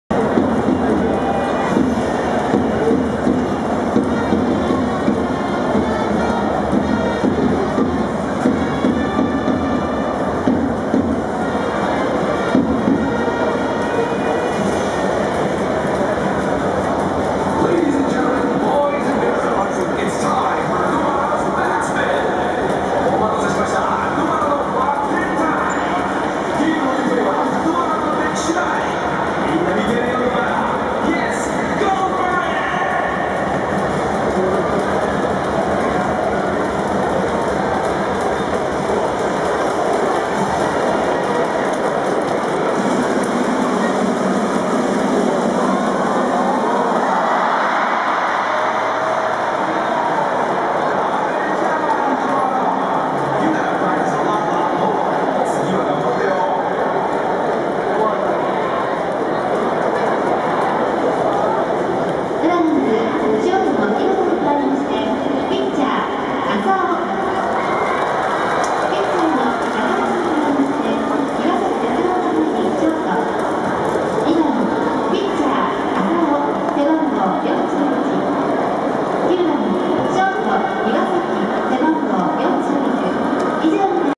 10/04/20 ドアラの占いバック転タイム
ドアラはただいま連敗中

今日の結果は？？

残念、今日も失敗でした

ドアラ、気持ちを切り替えてがんばれ！